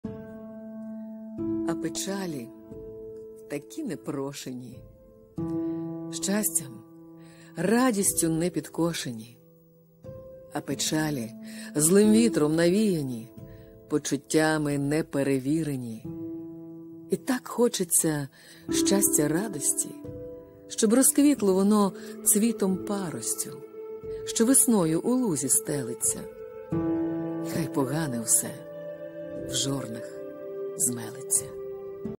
Олена Хижна. А печалi такi непрошенi....
вірш Олександра Хмельовського " А печалi такi непрошенi. " з книги поезiй "Небесна i земна" читає заслуженна артистка України Олена Хижна.